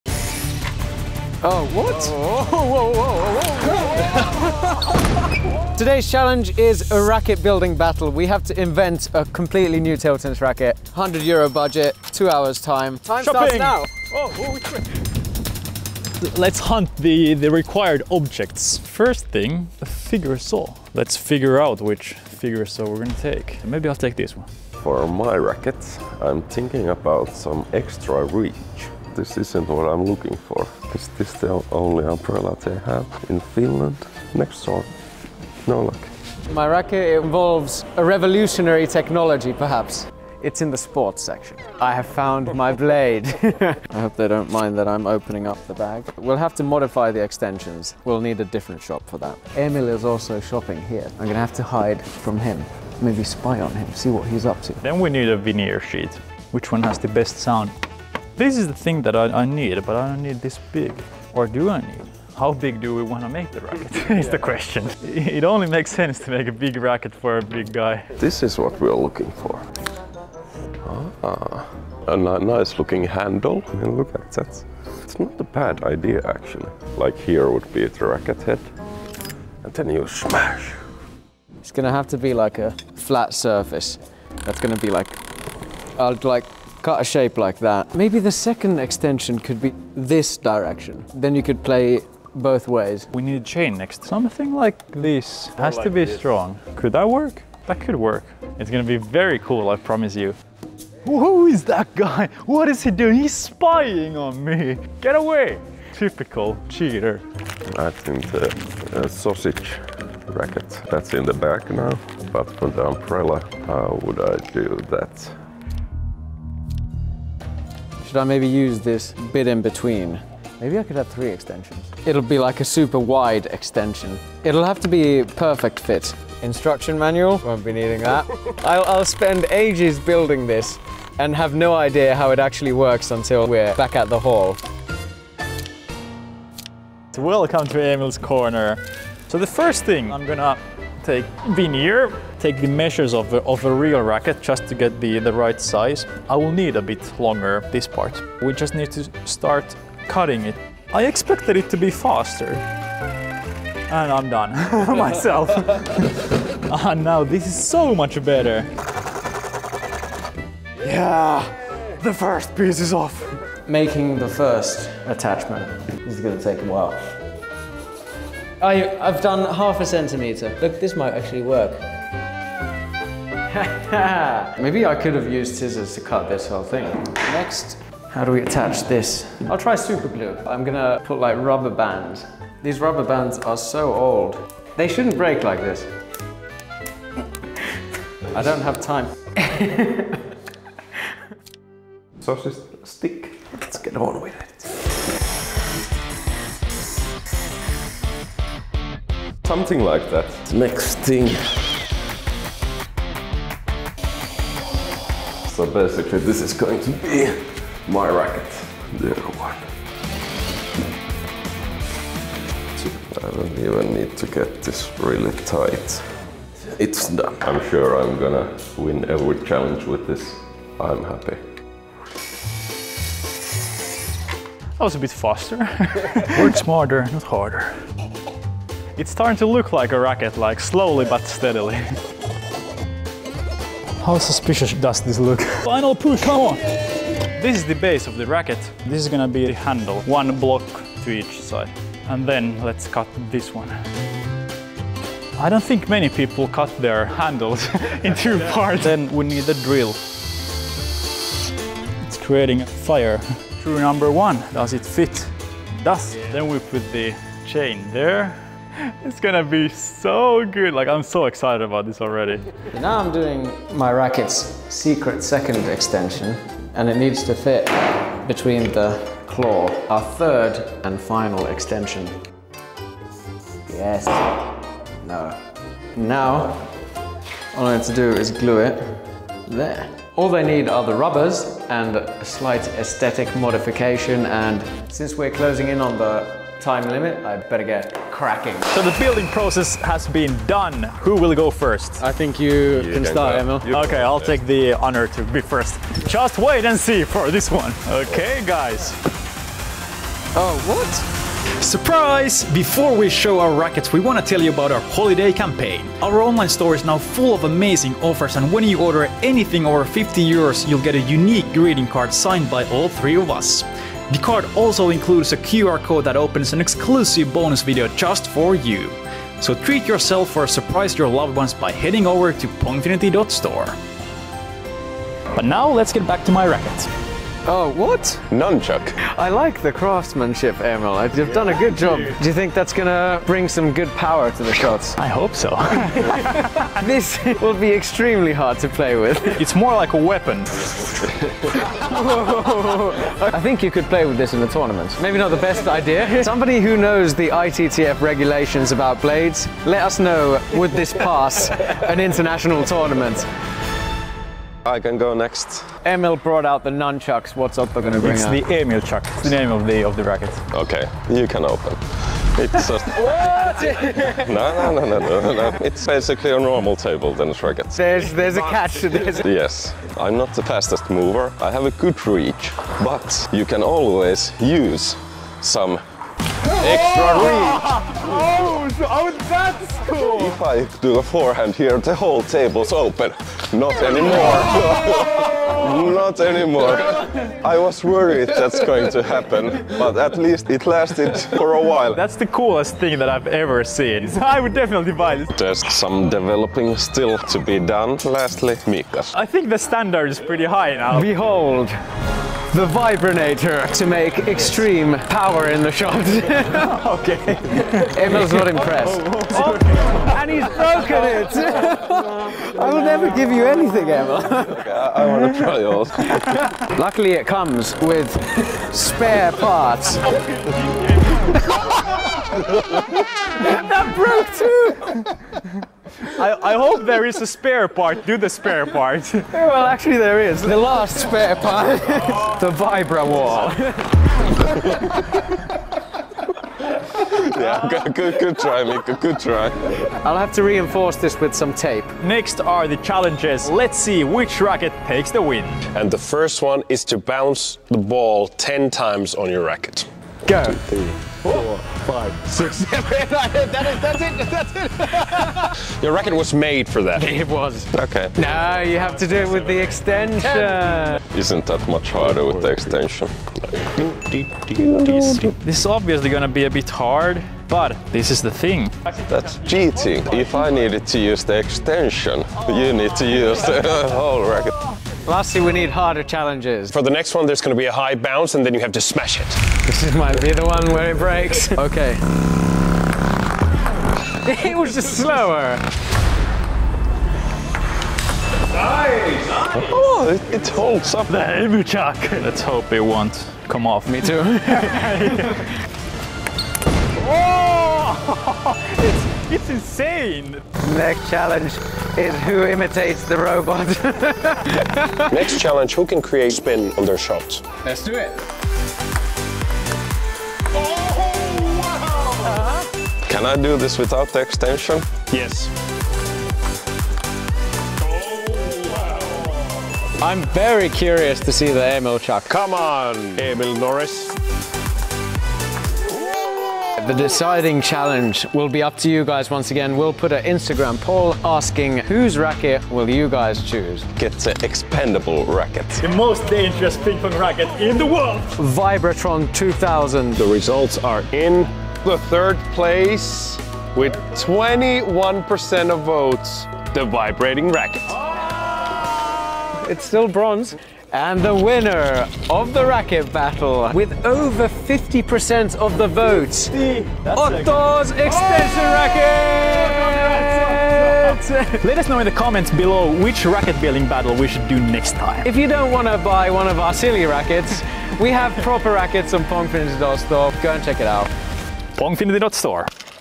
Ultimate Level Ping Pong
We can't believe how good our rackets turned out to be!